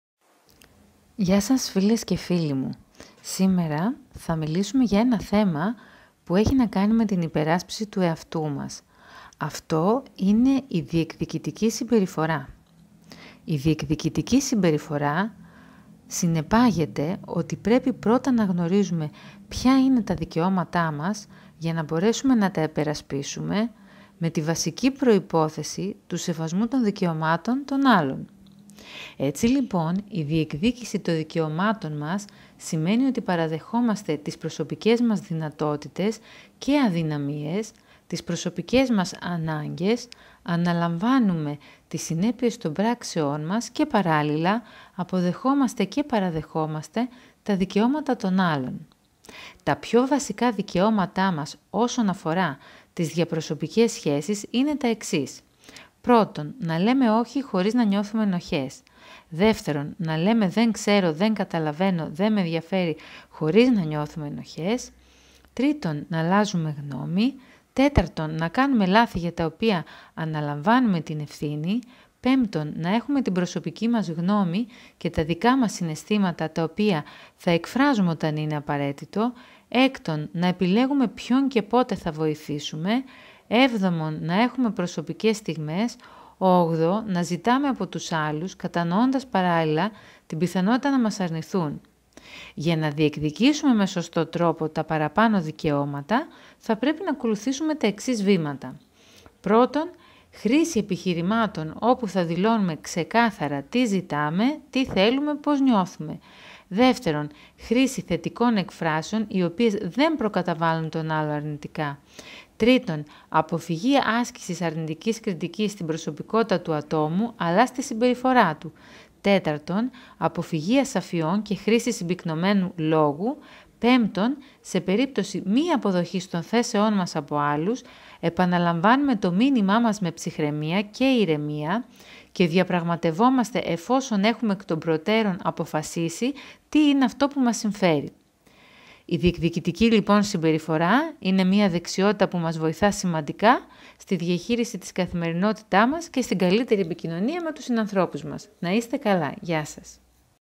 "Διεκδικητικότητα: Η τέχνη του να υπερασπίζω τον εαυτό μου"
Τί σημαίνει διεκδικητική συμπεριφορά;
Πώς υπερασπίζω τον εαυτό μου;
Ποια είναι τα βασικά δικαιώματα μας αναφορικά με τις διαπροσωπικές σχέσεις;
Πώς διεκδικούμε τα δικαιώματά μας με σωστό τρόπο;